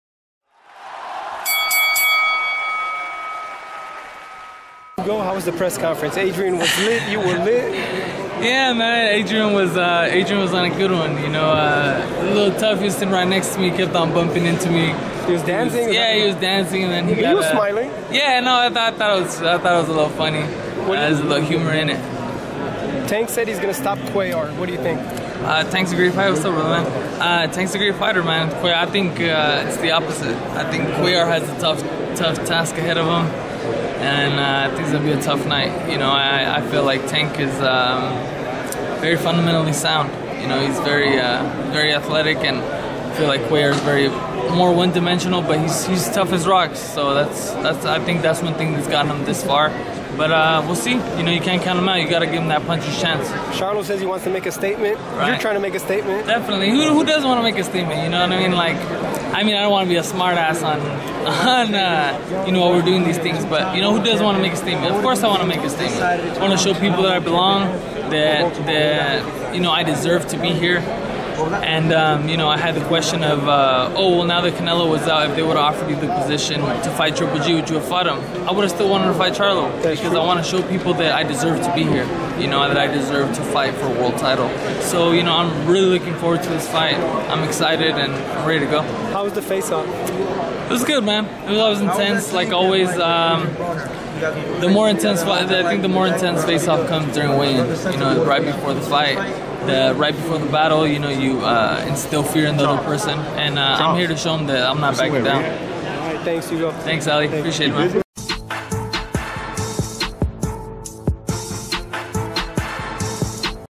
Hugo Centeno Reveals What Was It Like To Sit Near Adrien Broner At Presser EsNews Boxing
Hugo Centeno Reveals What Was It Like To Sit Near Adrien Broner At Presser